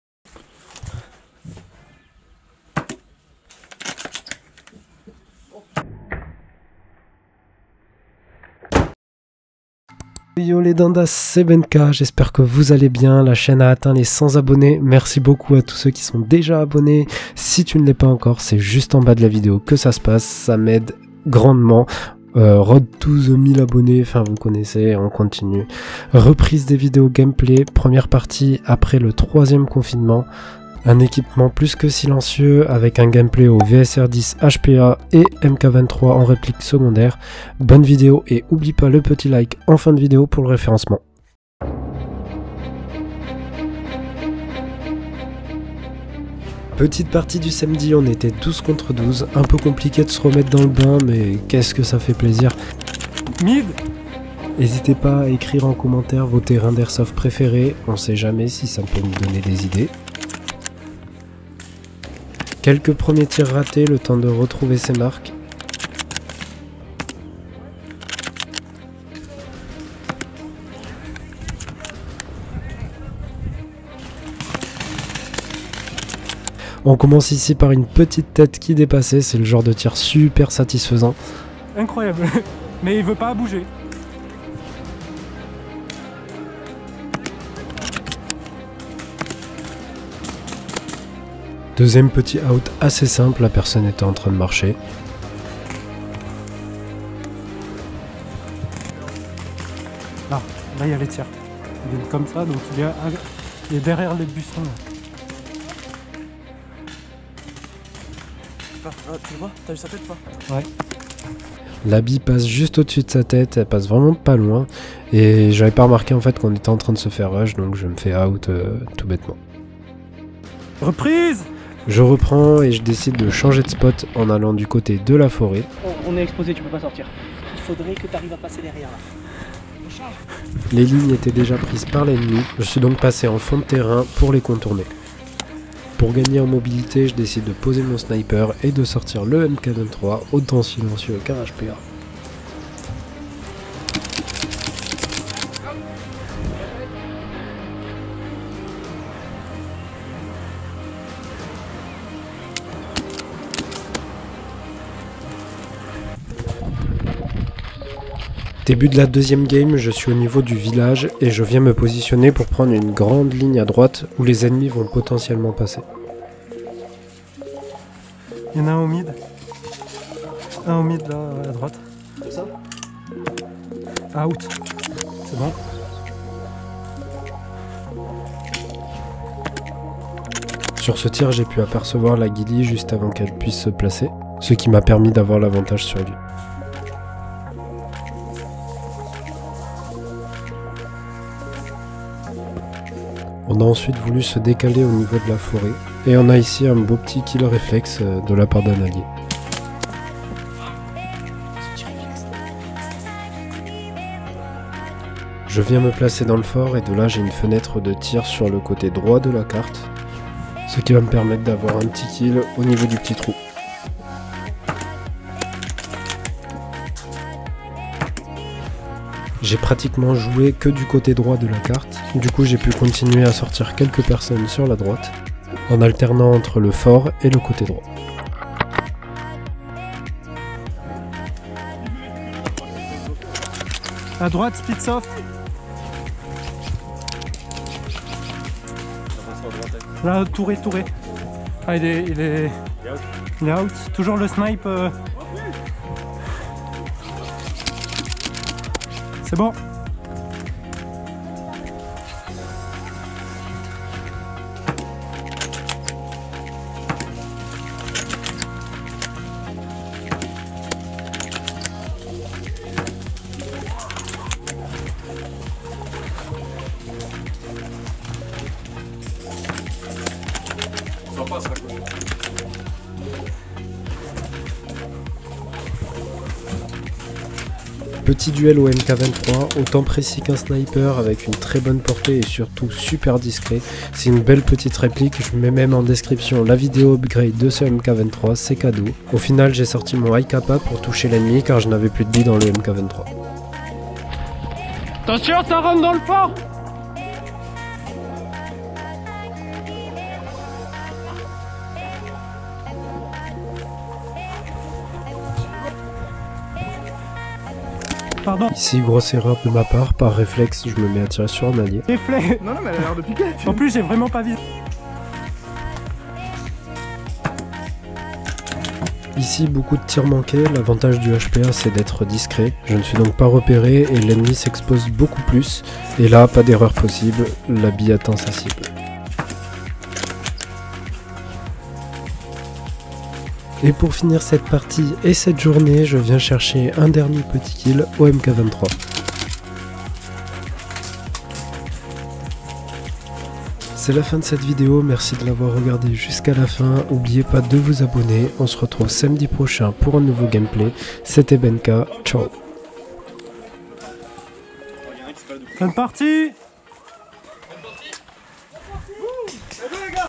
BNK Airsoft Gameplay - Les Répliques les plus SILENCIEUSES [ VSR10 HPA / MK23 ]
Hey les dindasses bienvenue sur une nouvelle vidéo d'airsoft, en espérant que le montage vous plaira.
Lieu:
Route de Quint, 31650 Saint-Orens-de-Gameville
Mon équipement :
VSR 10 HPA
Hicapa 5.1 TM